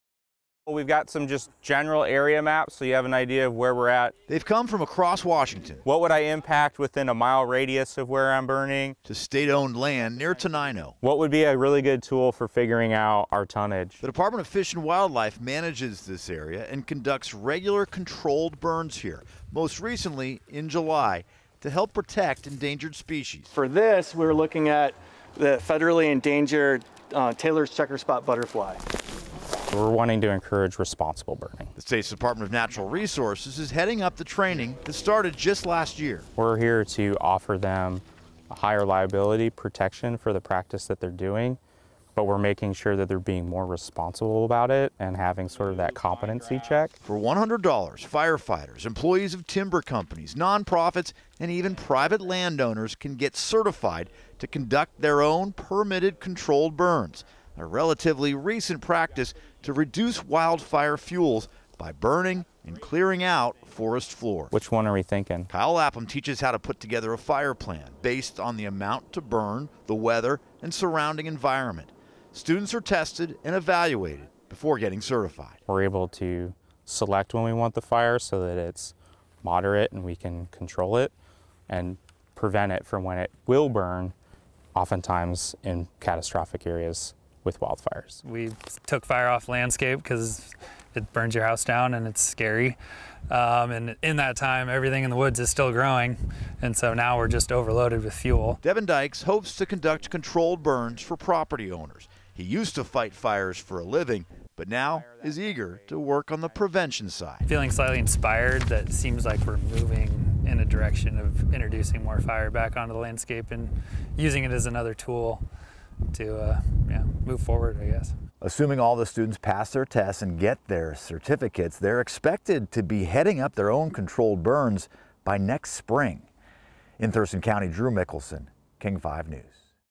Washington Department of Natural Resources offers trainings for controlled burns
Last year, the state started offering the training courses under legislation, passed in 2018, to boost the number of controlled burns across the state. The 2018 bill fought to establish a prescribed burn certification program at the department of natural resources.

“We’re wanting to encourage responsible burning,” said Kyle Lapham, the Department of Natural Resources’ Certified Burner program manager.

Lapham said prescribed burns — events planned around weather and conditions — are an effective way to reduce fuels in the forest, as well as improve the environment for endangered species. He teaches students how to make a fire plan, which is based on how much of an area to burn, the weather and the surrounding environment.